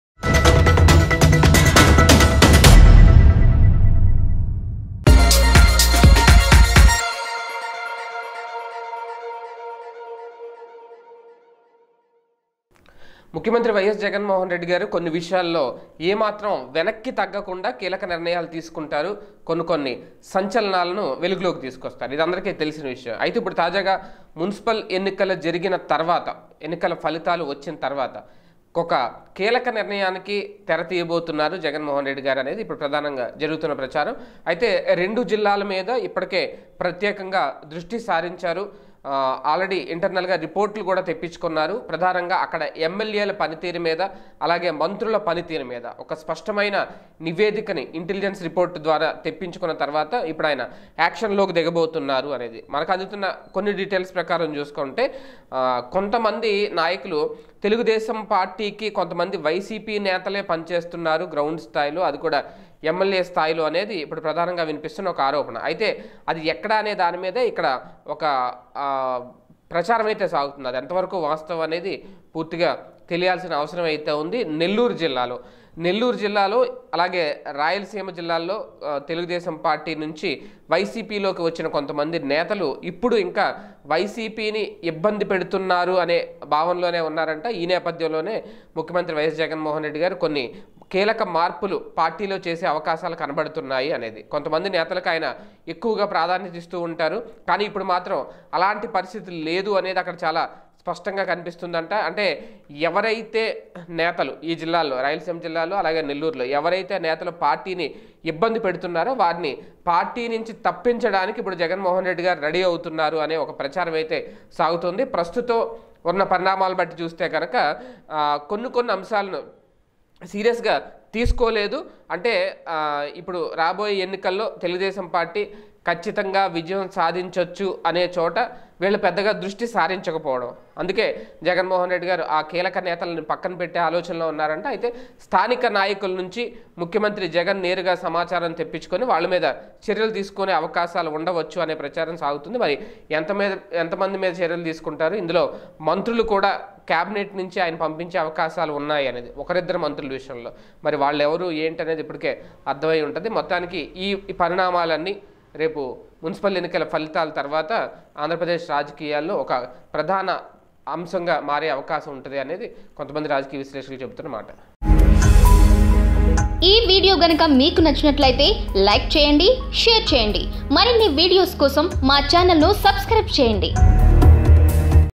CM Ys Jagan Action On YSRCP MLA's |బిగ్ బ్రేకింగ్: ఈ రెండు జిల్లాల ఎమ్మెల్యేలపై జగన్ వేటు?|PDTV News
బిగ్ బ్రేకింగ్: ఈ రెండు జిల్లాల ఎమ్మెల్యేలపై జగన్ వేటు..?